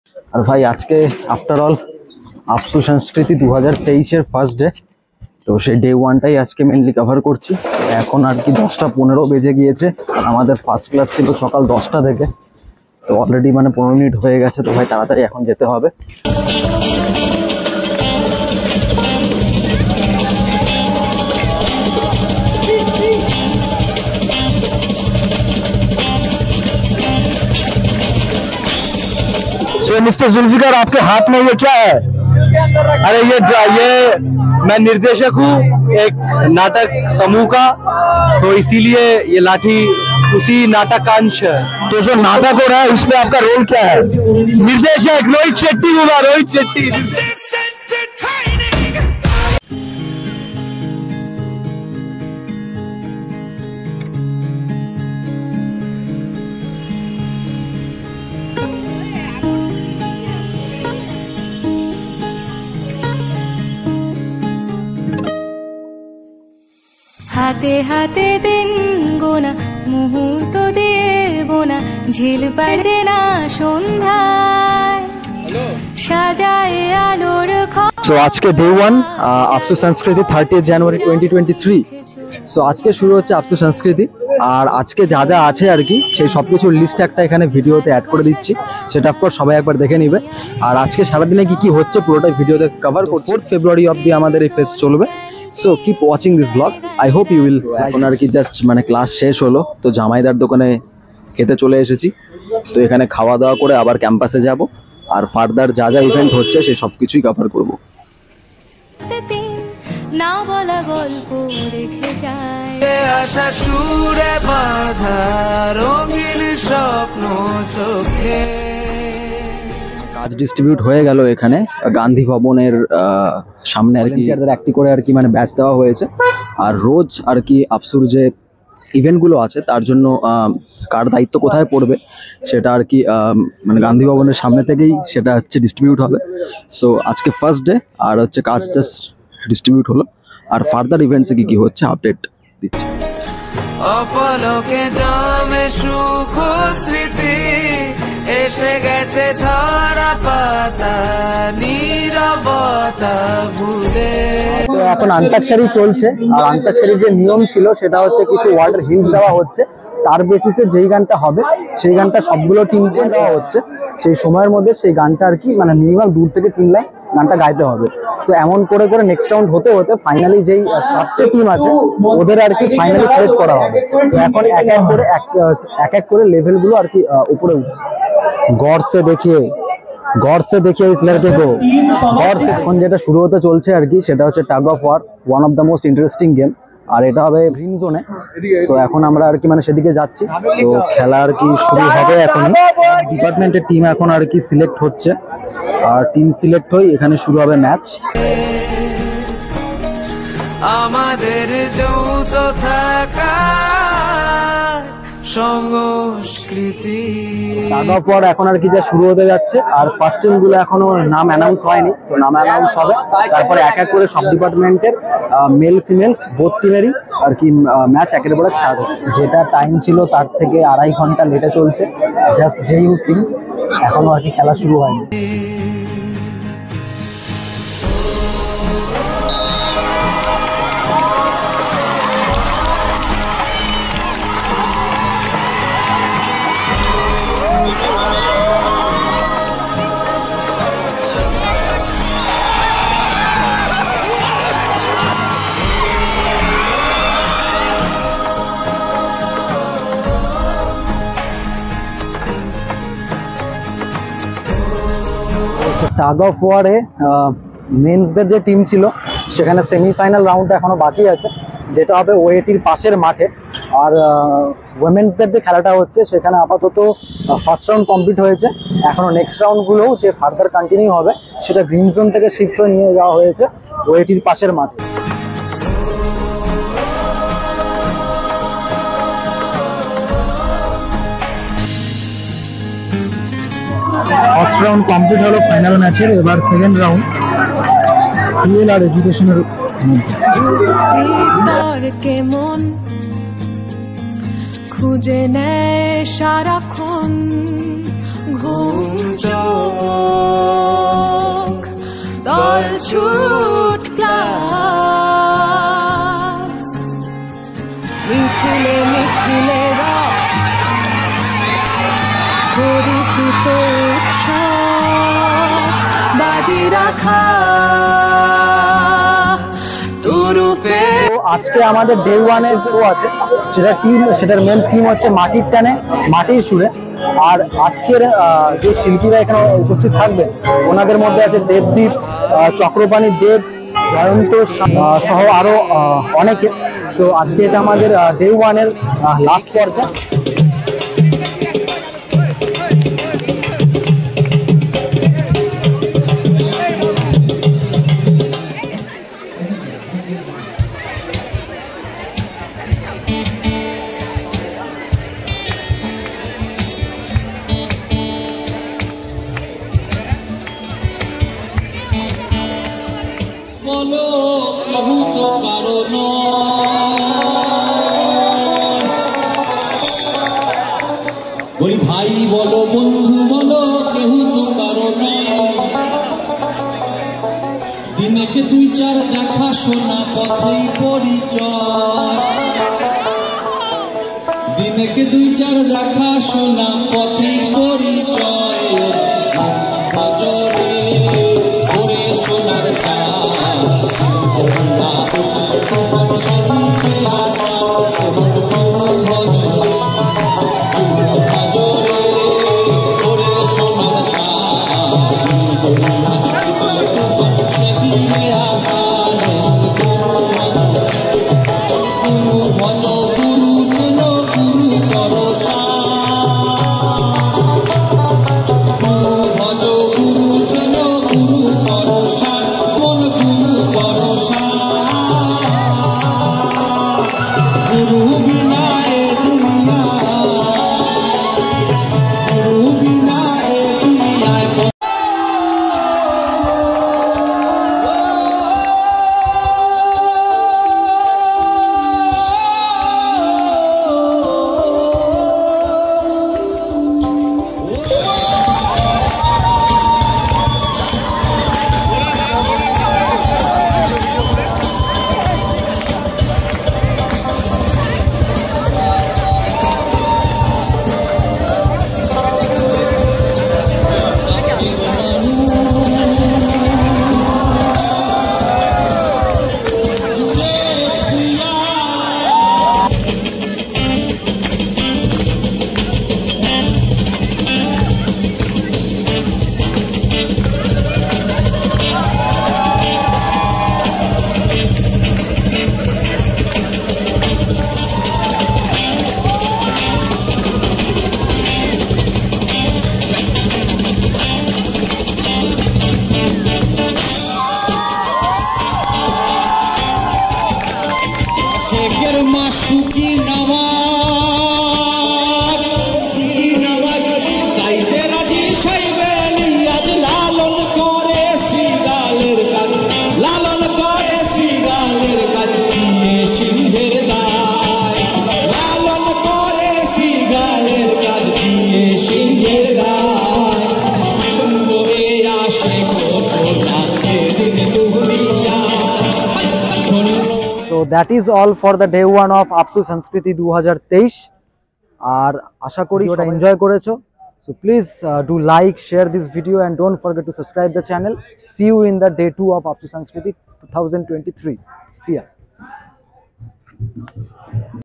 SANSKRITI 2023 | JADAVPUR UNIVERSITY | Arts Faculty Students Union (AFSU) | DAY 1
Day 1 of AFSU SANSKRITI 2023. All events have been covered with in this vlog. Hope you'd enjoy watching this. Thank you.